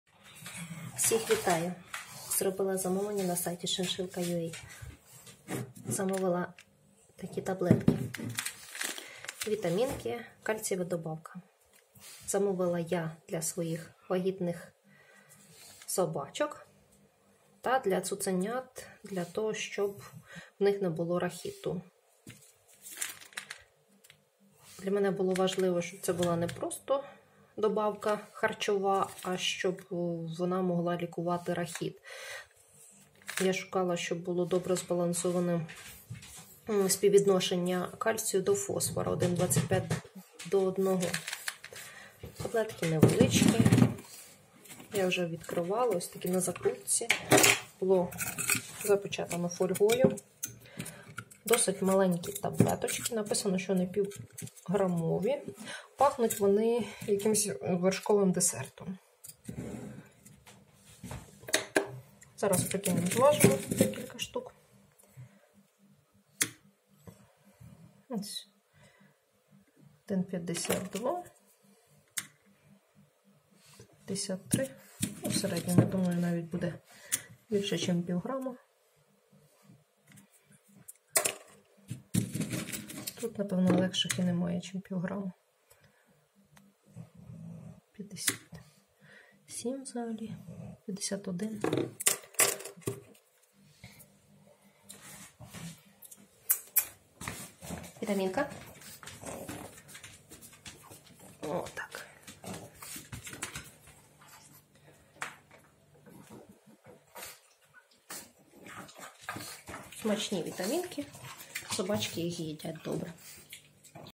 8in1 Excel Calcium - кальциевая добавка для собак
8in1 Excel Calcium - добавка для щенков и взрослых собак, содержащая кальций и фосфор, необходимые для укрепления зубов и костей. Незаменима для щенков в период активации роста, а также для сук во время беременности и лактации. Витамин D3, входящий в состав препарата, способствует оптимальному усвоению кальция.